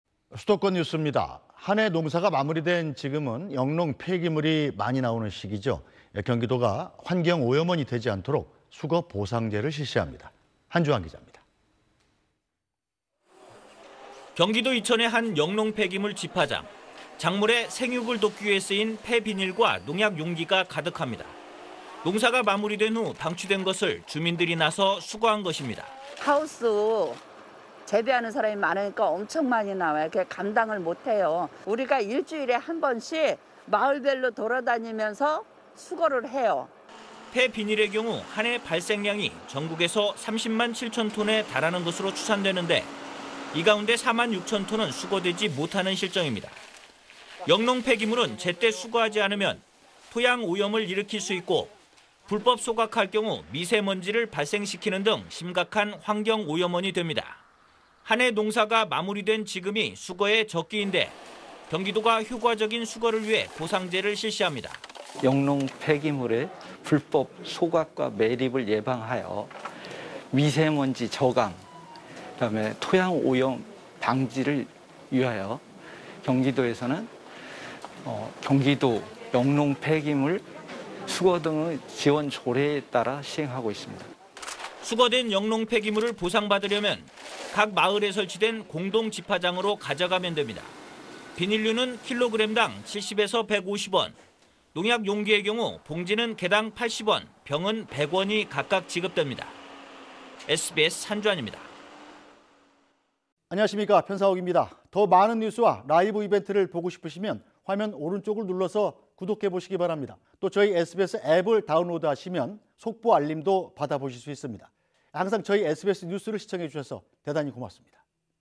영농 폐기물 수거하면 보상금 준다…경기도 보상제 실시 / SBS
한 해 농사가 마무리된 지금은 영농 폐기물이 많이 나오는 시기죠. 경기도가 환경 오염원이 되지 않도록 수거 보상제를 실시합니다.
한주한 기자입니다.